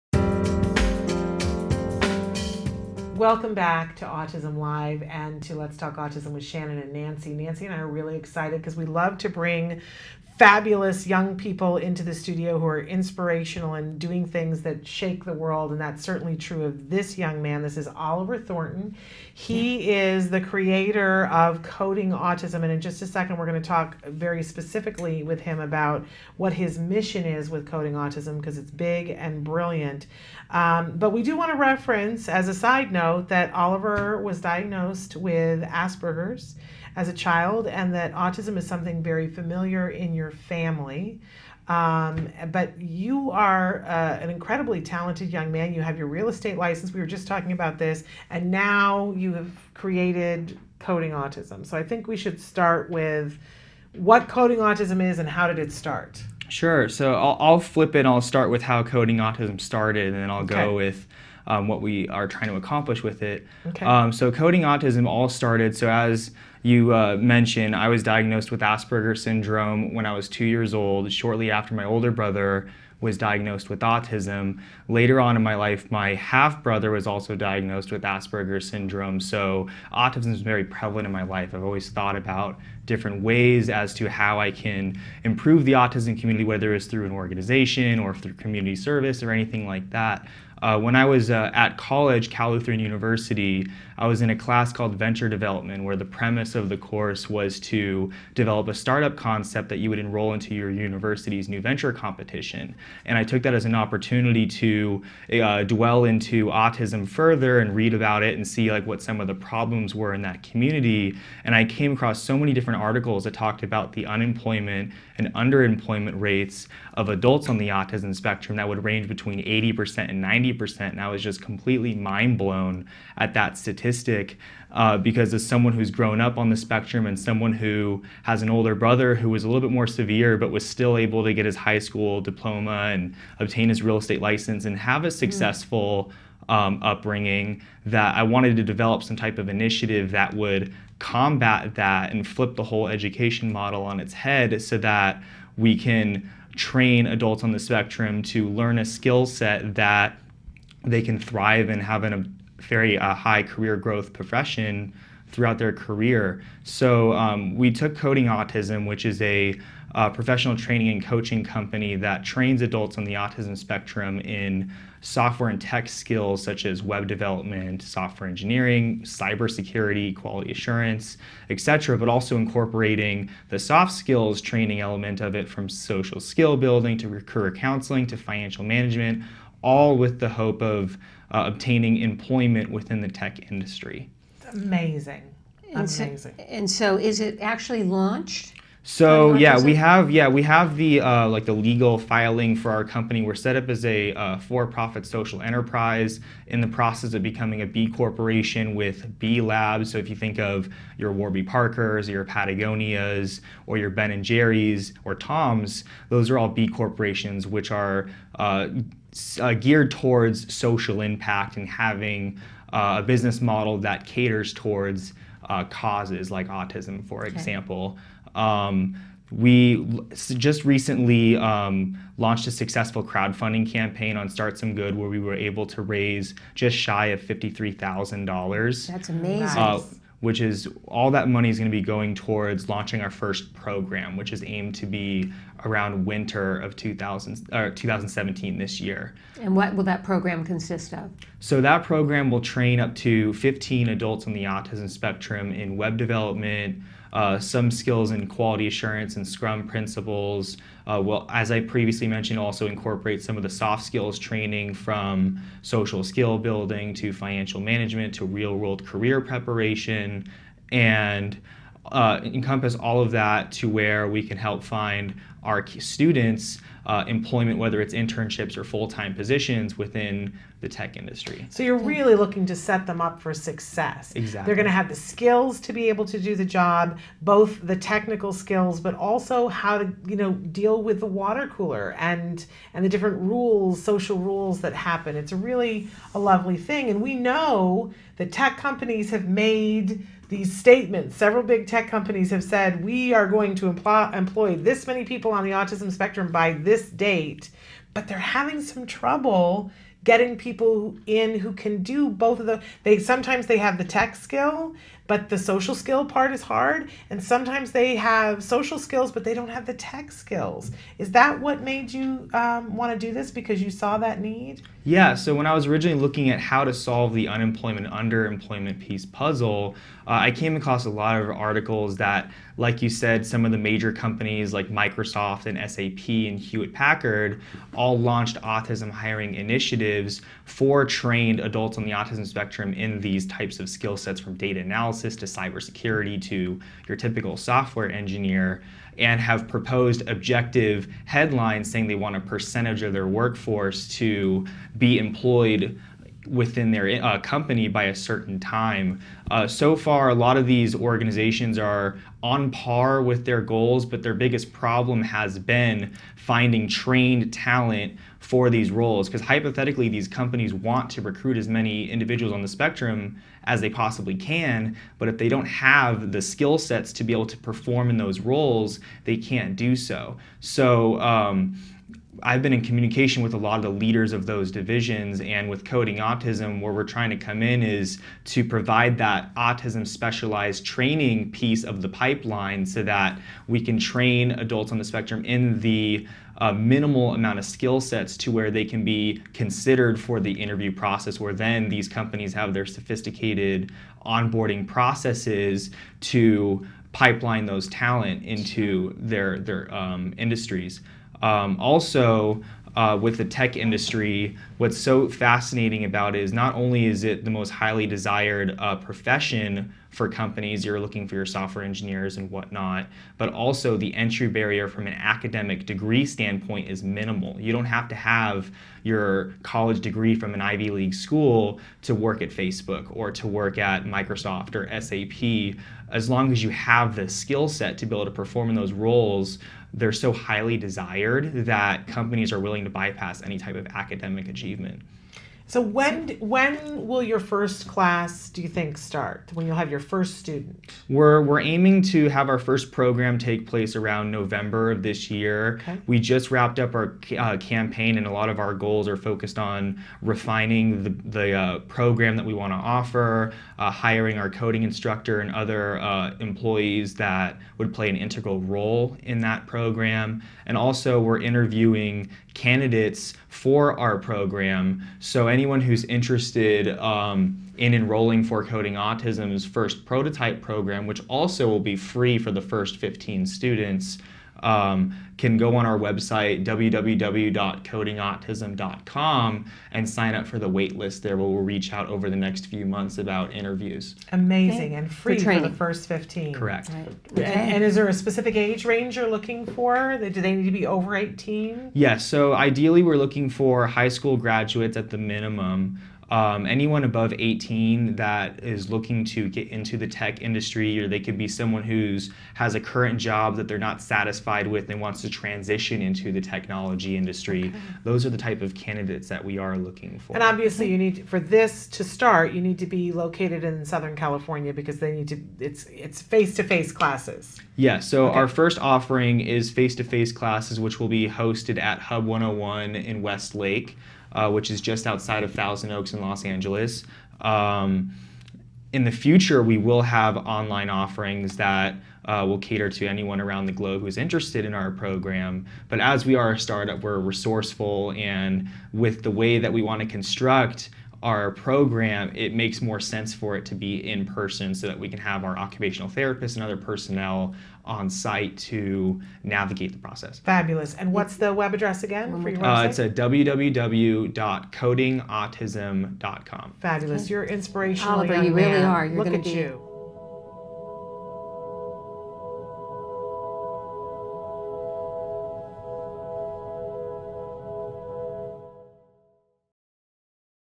Coding Autism- Oliver Thornton
Shannon and Nancy speak to Coding Autism's Oliver Thornton  about how coding skills can help the new generation of young adults on the spectrum.